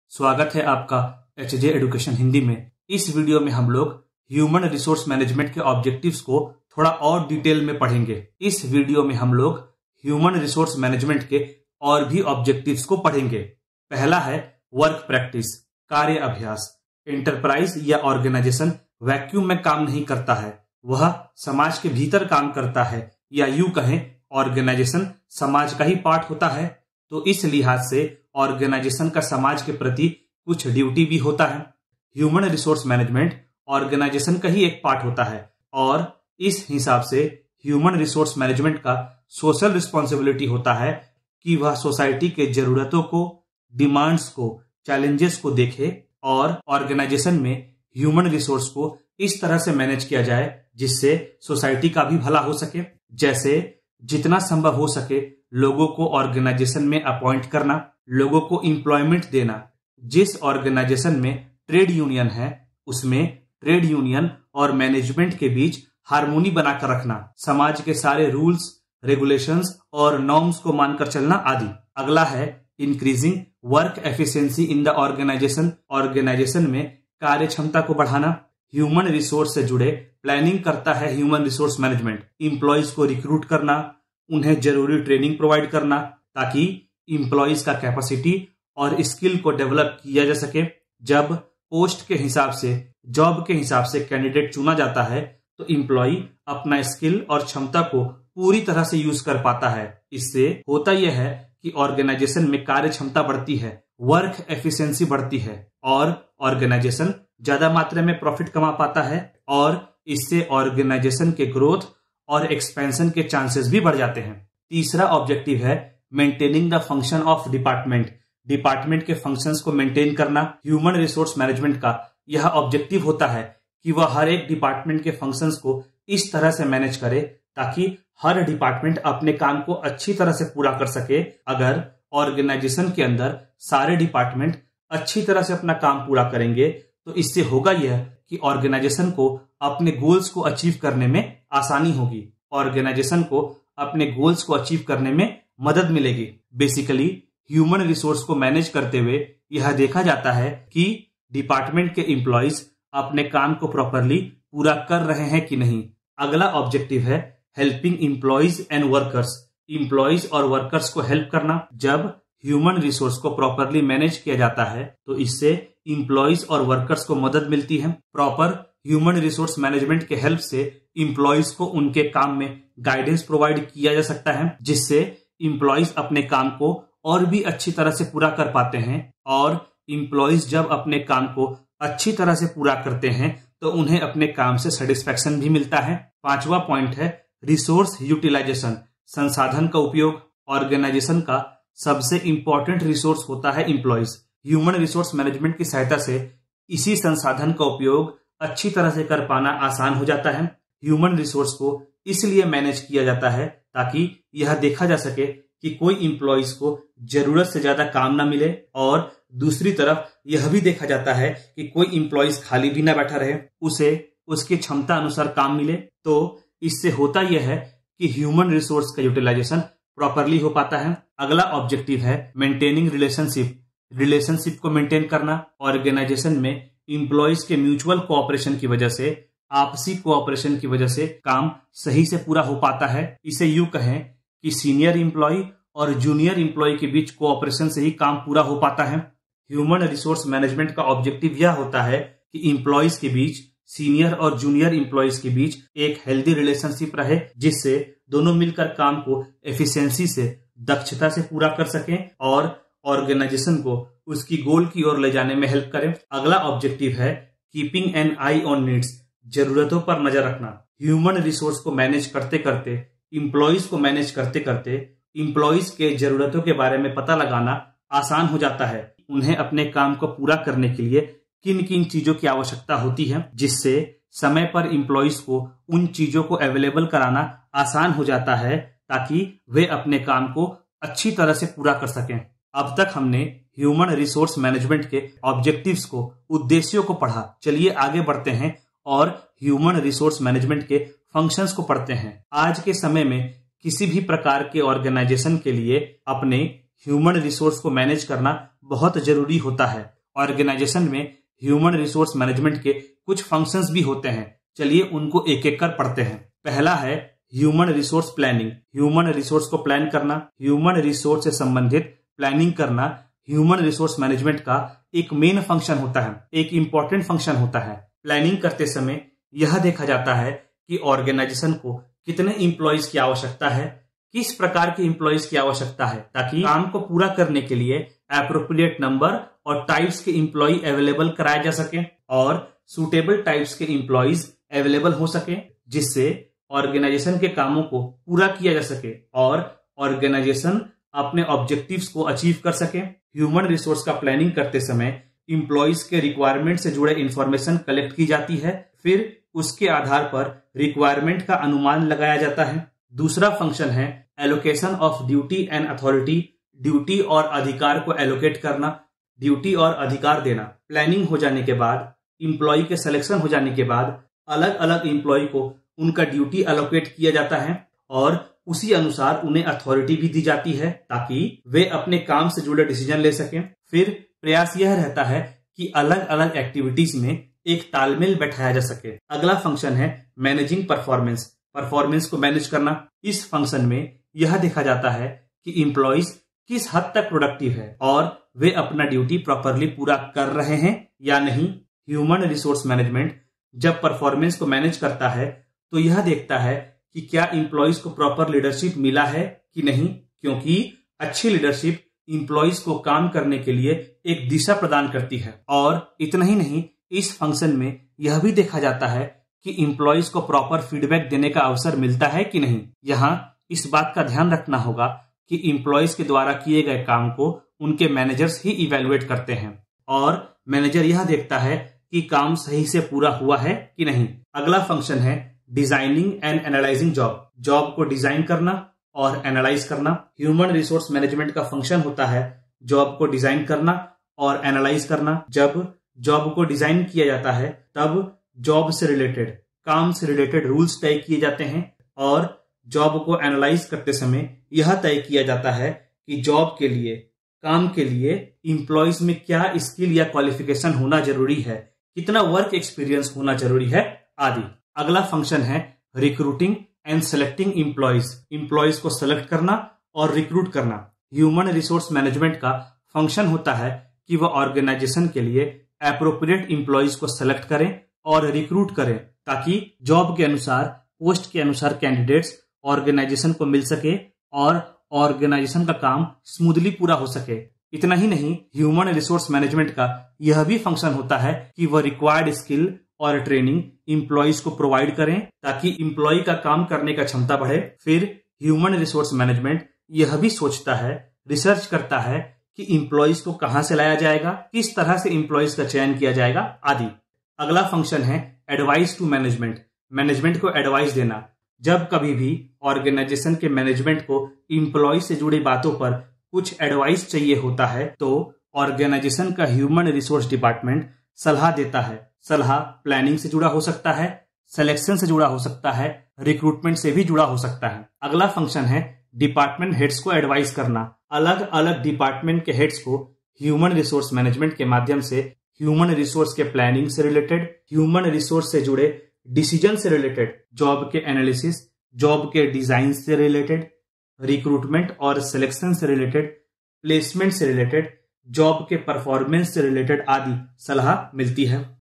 Objectives of Human Resource Management | Functions of Human Resource Management | hrm
The primary objective of Human Resource Management is to ensure the availability of competent workforce (with respect to the work) for the organisation so that the organisation can achieve it's goals. In this video we will study -

1. Some more objectives of Human Resource Management
2. Functions of Human resource management